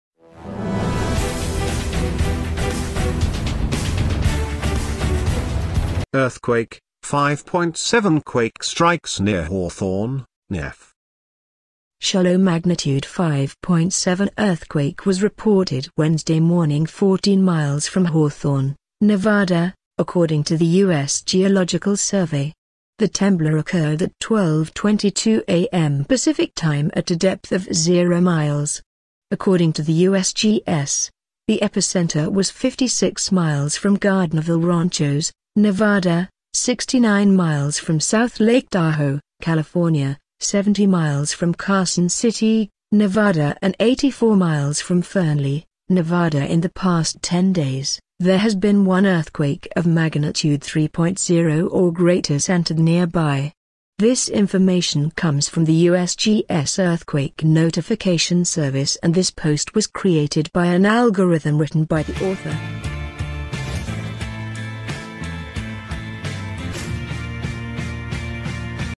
Earthquake: 5.7 quake strikes near Hawthorne, Nev - Updates
shallow magnitude 5.7 earthquake was reported Wednesday morning 14 miles from Hawthorne, Nev., according to the U.S. Geological Survey. The temblor occurred at 12:22 a.m. Pacific time at a depth of 0 miles.